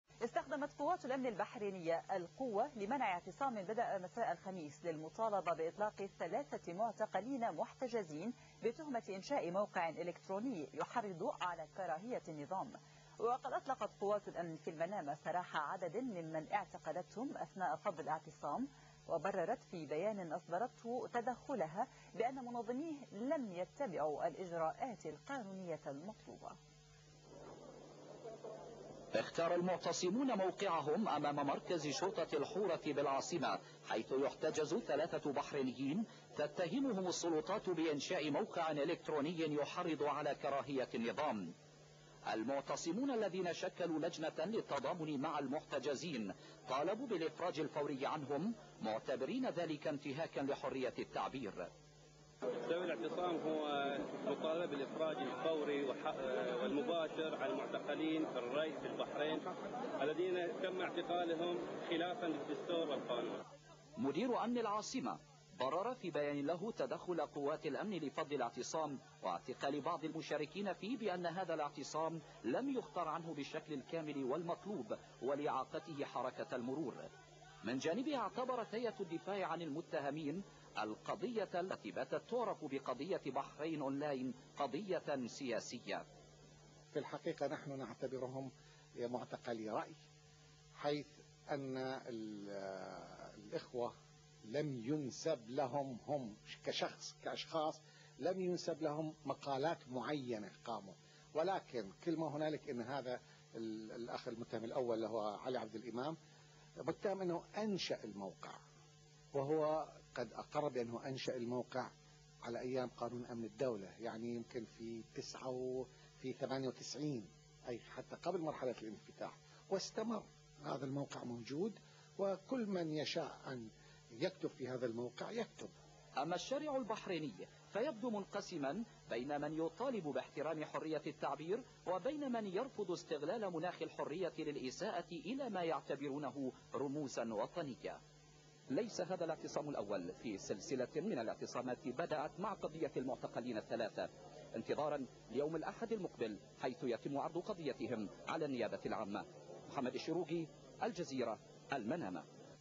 Blogger in Bahrain arrested - 1
Bahraini blogger Ali Abdulemam arrested by Bahrain government. Riot police shut down a peaceful demonstration.